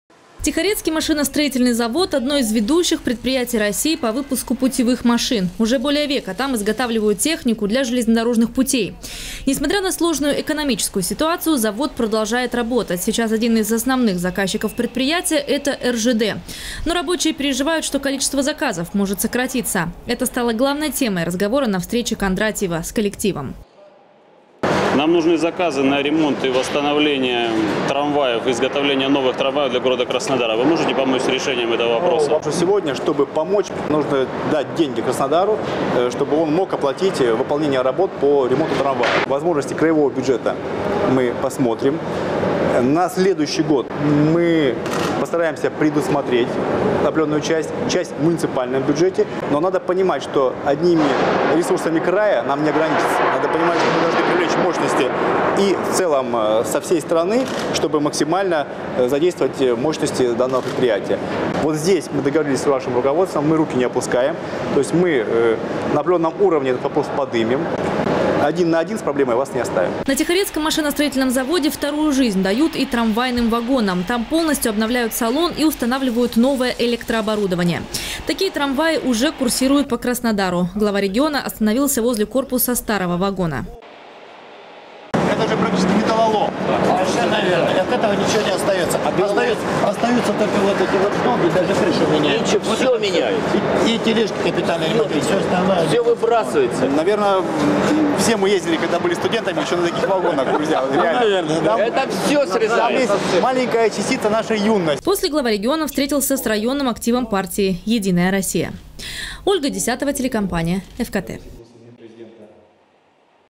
Промышленным предприятиям Кубани поддержку окажет бюджет региона. Новости Сочи Эфкате
Об этом рассказал Вениамин Кондратьев на встрече с рабочими Тихорецкого машиностроительного завода. Его руководитель края посетил в рамках рабочей поездки в район.
Сочи. Новости.
Телеканал Эфкате.